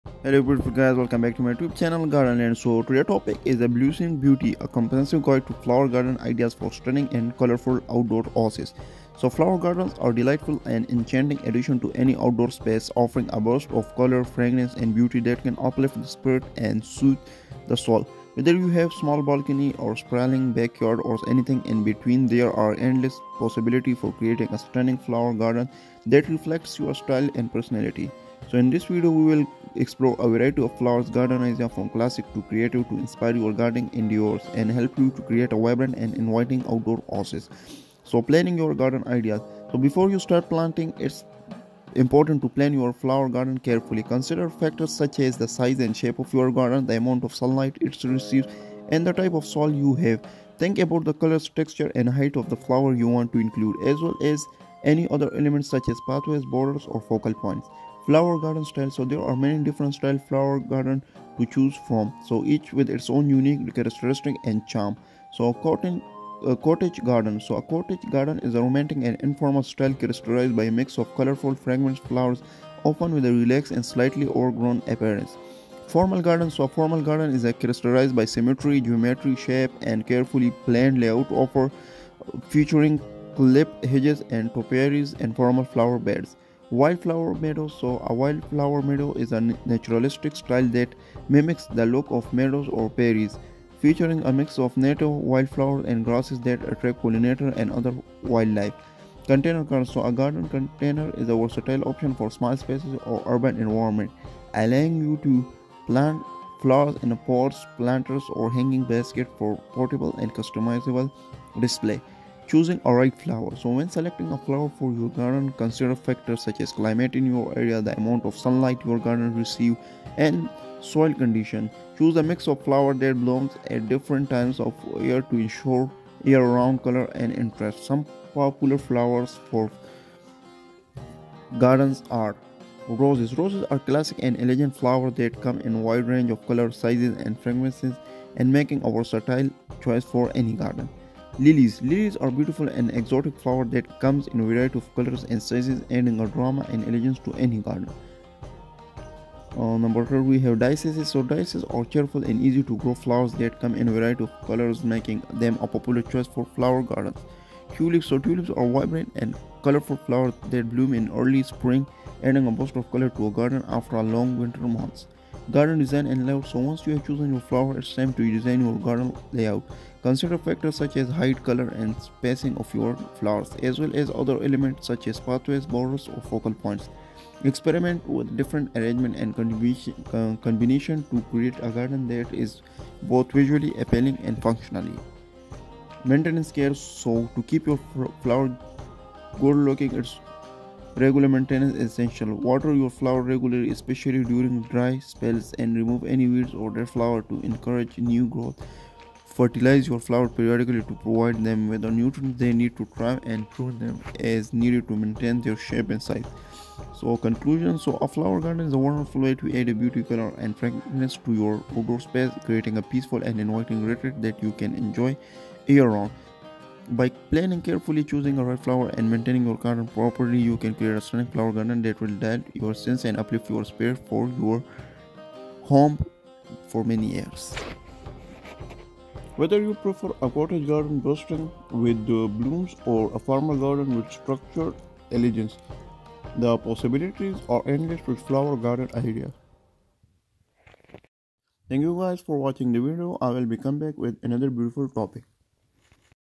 Blooms and Beauty: A Comprehensive Guide to Creating a Stunning Flower Garden with Inspiring Ideas
Title: "Blossoming Beauty: A Comprehensive Guide to Flower Garden Ideas for a Stunning and Colorful Outdoor Oasis"

Introduction

Flower gardens are a delightful and enchanting addition to any outdoor space, offering a burst of color, fragrance, and beauty that can uplift the spirit and soothe the soul. Whether you have a small balcony, a sprawling backyard, or anything in between, there are endless possibilities for creating a stunning flower garden that reflects your style and personality. In this comprehensive guide, we will explore a variety of flower garden ideas, from classic to creative, to inspire your gardening endeavors and help you create a vibrant and inviting outdoor oasis.

1. Planning Your Flower Garden

Before you start planting, it's important to plan your flower garden carefully. Consider factors such as the size and shape of your garden, the amount of sunlight it receives, and the type of soil you have. Think about the colors, textures, and heights of the flowers you want to include, as well as any other elements such as pathways, borders, or focal points.

2. Flower Garden Styles

There are many different styles of flower gardens to choose from, each with its own unique characteristics and charm. Some popular styles include:

Cottage Garden: A cottage garden is a romantic and informal style characterized by a mix of colorful, fragrant flowers, often with a relaxed and slightly overgrown appearance.
Formal Garden: A formal garden is characterized by symmetry, geometric shapes, and a carefully planned layout, often featuring clipped hedges, topiaries, and formal flower beds.
Wildflower Meadow: A wildflower meadow is a naturalistic style that mimics the look of a meadow or prairie, featuring a mix of native wildflowers and grasses that attract pollinators and other wildlife.
Container Garden: A container garden is a versatile option for small spaces or urban environments, allowing you to plant flowers in pots, planters, or hanging baskets for a portable and customizable display.
3. Choosing the Right Flowers

When selecting flowers for your garden, consider factors such as the climate in your area, the amount of sunlight your garden receives, and the soil conditions. Choose a mix of flowers that bloom at different times of the year to ensure year-round color and interest. Some popular flowers for flower gardens include:

Roses: Roses are classic and elegant flowers that come in a wide range of colors, sizes, and fragrances, making them a versatile choice for any garden.
Lilies: Lilies are beautiful and exotic flowers that come in a variety of colors and sizes, adding drama and elegance to any garden.
Daisies: Daisies are cheerful and easy-to-grow flowers that come in a variety of colors, making them a popular choice for flower gardens.
Tulips: Tulips are vibrant and colorful flowers that bloom in early spring, adding a burst of color to the garden after the long winter months.
4. Garden Design and Layout

Once you've chosen your flowers, it's time to design your garden layout. Consider factors such as the height, color, and spacing of your flowers, as well as any other elements such as pathways, borders, or focal points. Experiment with different arrangements and combinations to create a garden that is both visually appealing and functional.

5. Maintenance and Care

To keep your flower garden looking its best, regular maintenance is essential. Water your flowers regularly, especially during dry spells, and remove any weeds or dead flowers to encourage new growth. Fertilize your flowers periodically to provide them with the nutrients they need to thrive, and prune them as needed to maintain their shape and size.

6. Conclusion

A flower garden is a wonderful way to add beauty, color, and fragrance to your outdoor space, creating a peaceful and inviting retreat that you can enjoy year-round. By planning carefully, choosing the right flowers, and maintaining your garden properly, you can create a stunning flower garden that will delight your senses and uplift your spirits for years to come. Whether you prefer a cottage garden bursting with blooms or a formal garden with structured elegance, the possibilities are endless with flower garden ideas.